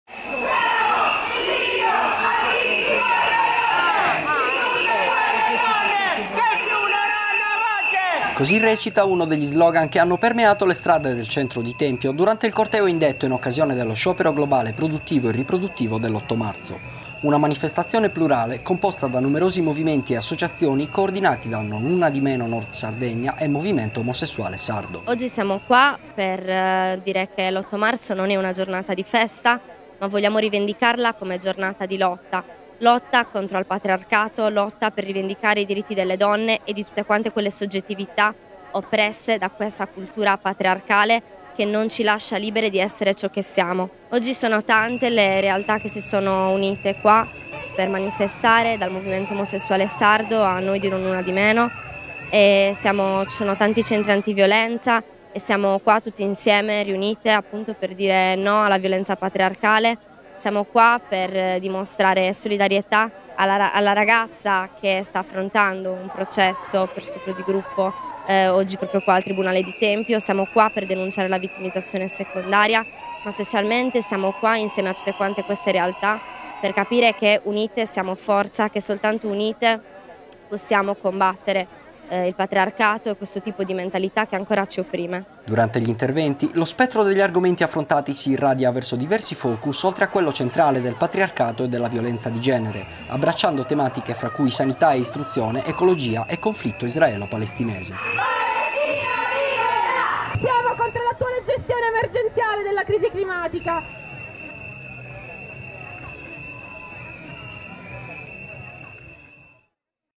Tempio, manifestazione pacifica all'insegna del rifiuto della violenza di genere
Anche a Tempio l’8 marzo è all’insegna del rifiuto della violenza di genere e della cultura patriarcale: questi e altri temi sono stati al centro della manifestazione pacifica che ha riunito numerosi movimenti e associazioni coordinati da Non una di meno Nord Sardegna e Movimento Omosessuale Sardo.